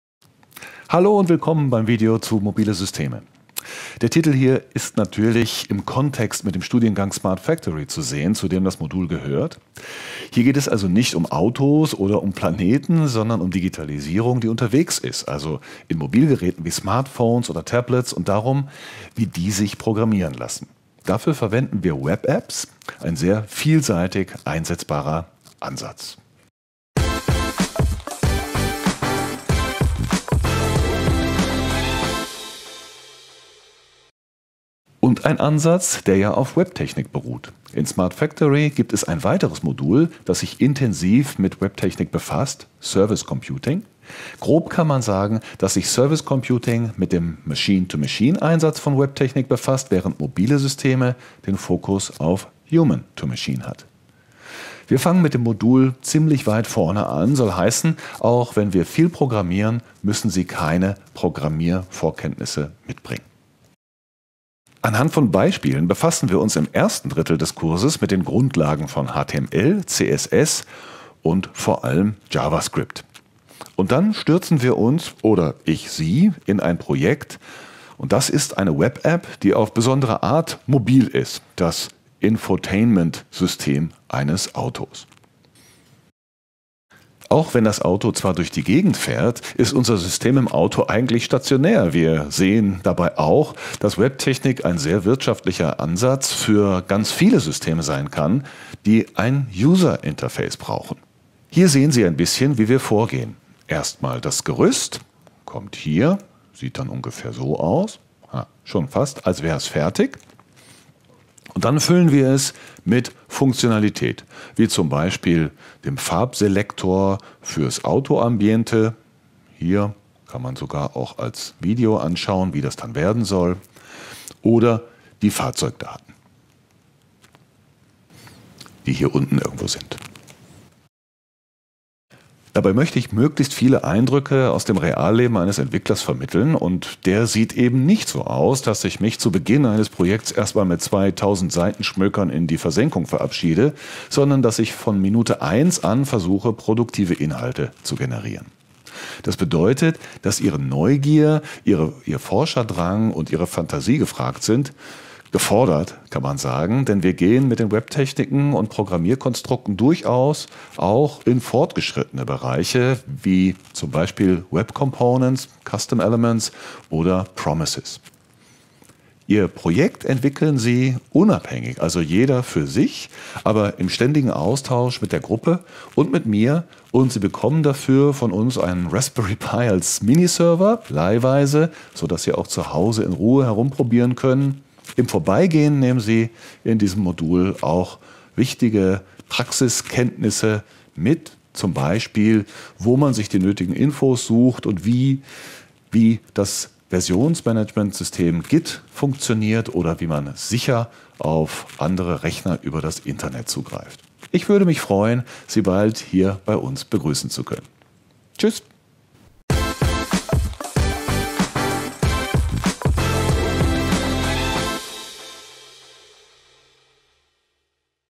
Digitalisierung, die unterwegs ist: Unser Kurs "Mobile Systeme"
Professor Dr. Ben Marx erklärt, was in seinem Kursmodul "Mobile Systeme" drankommt. Da ihr den Kurs im Masterstudiengang Smart Factory (SFM) belegen könnt, geht es also nicht um Autos oder Planeten, sondern um Digitalisierung, die unterwegs ist, wie bei Smartphones oder Tablets. Zum Programmieren solcher Endgeräte verwenden wir WebApps, das geht auch ohne Vorwissen in Programmierung. Hier gehts zum Masterstudiengang Smart Factory